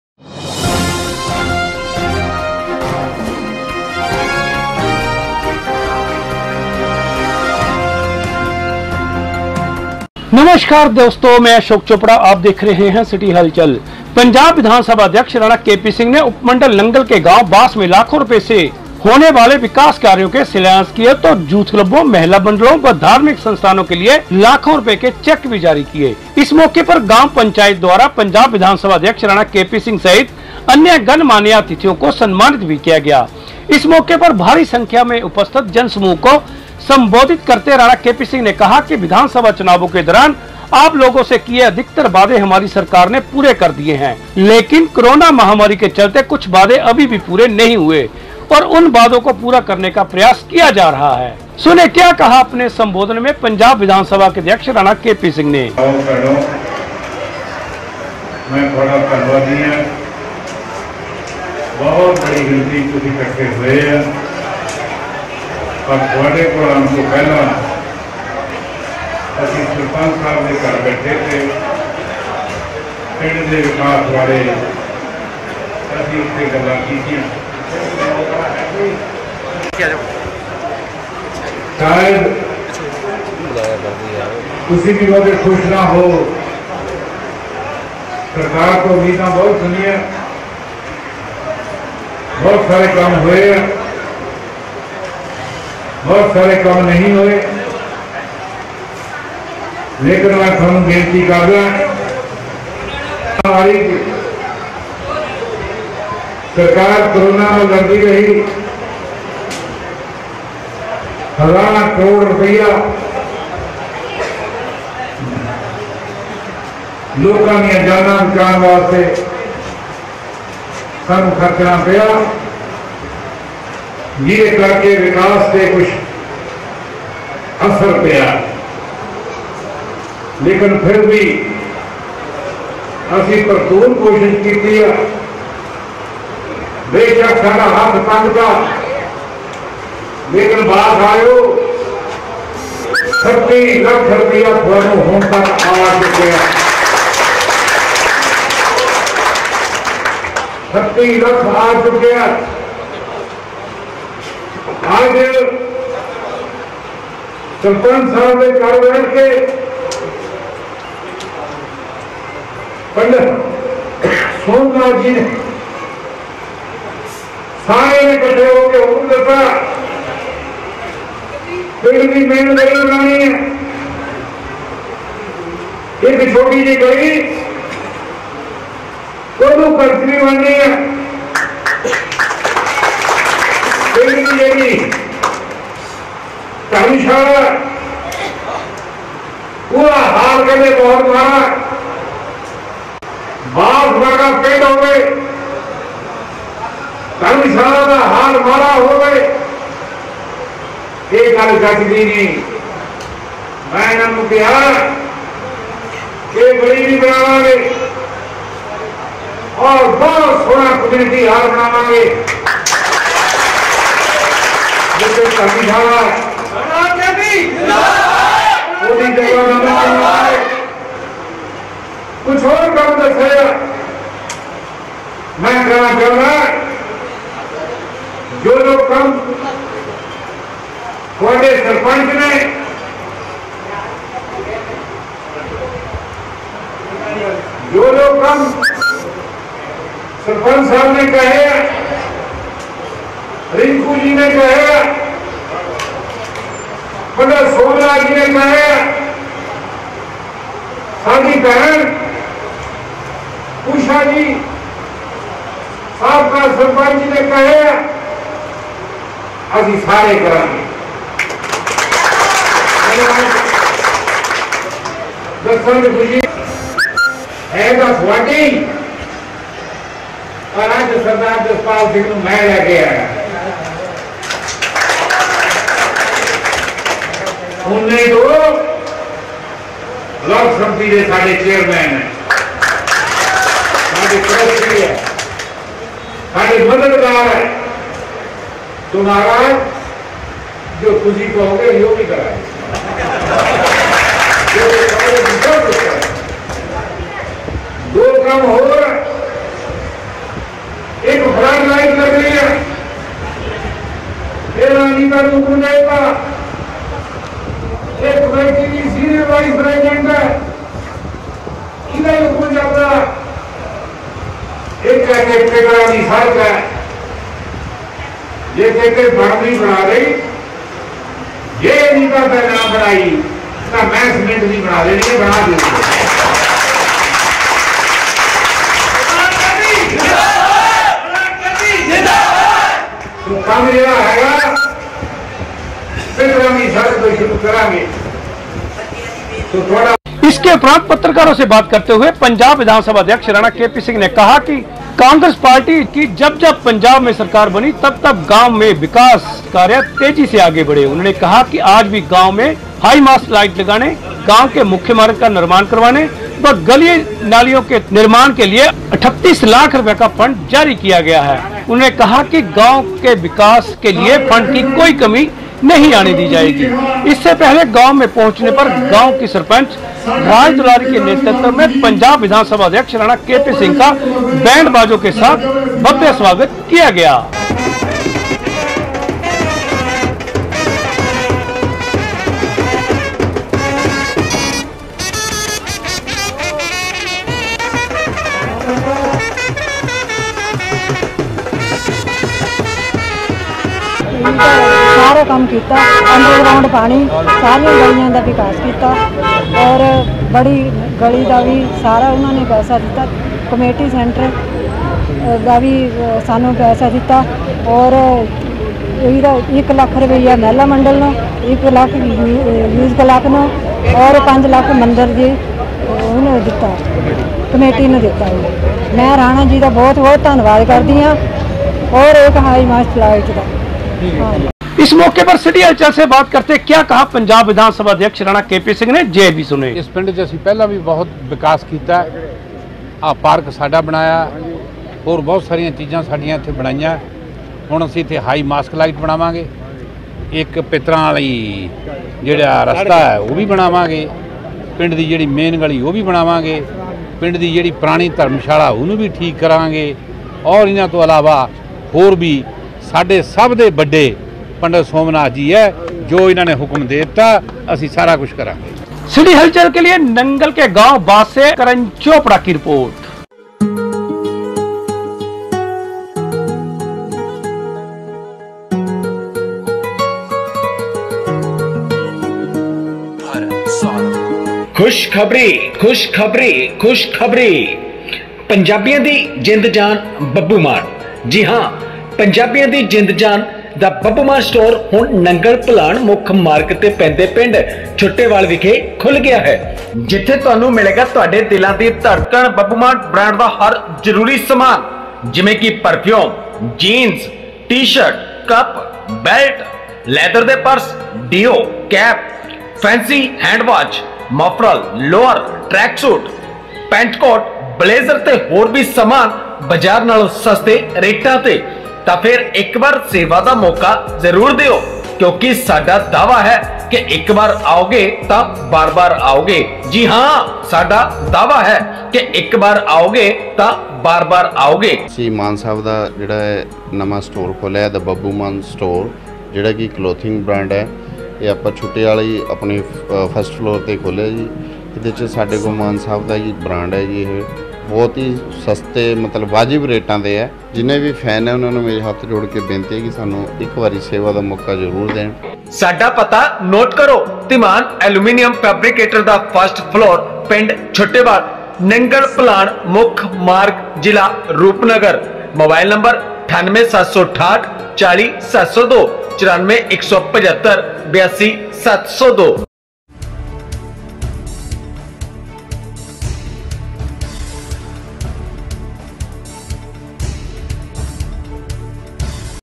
उपमंडल नंगल के गांव बास में राणा के सिंह ने लाखों के विकास कार्यो के किये उद्धघाटन देखे इस रिपोर्ट
उपमंडल नंगल के गांव बास में राणा के सिंह ने लाखों के विकास कार्यो के किये उद्धघाटन देखे इस रिपोर्ट में